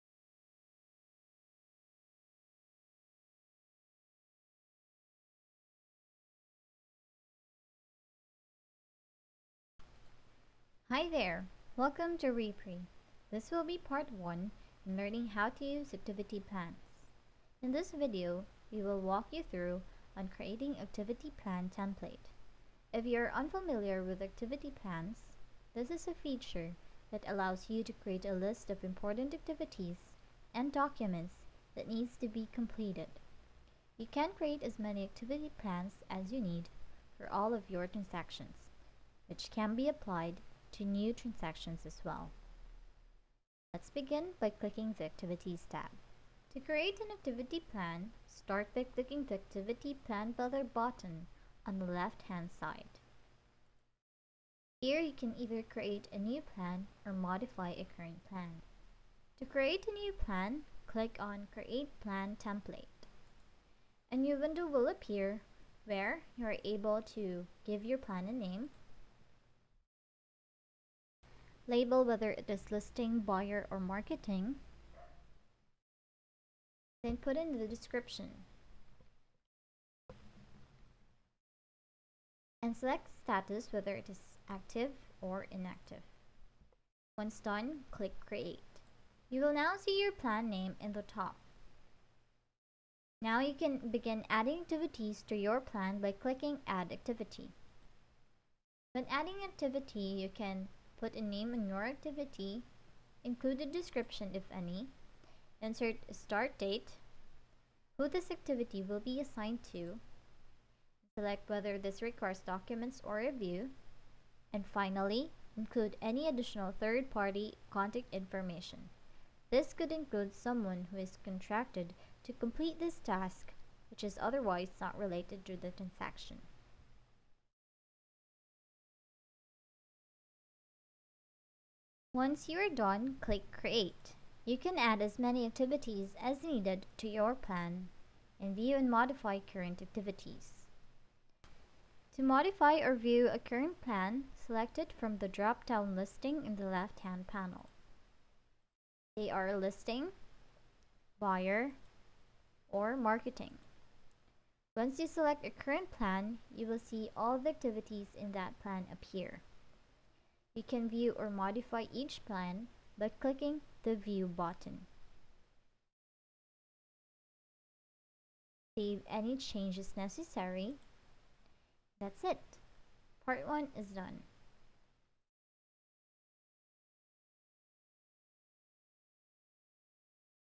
Activity Plans Part 1: Creating Templates
Part 1 outlines what the Activity feature is, and how to use it. Learn how to create an Activity Plan, and stay organized with all your listings.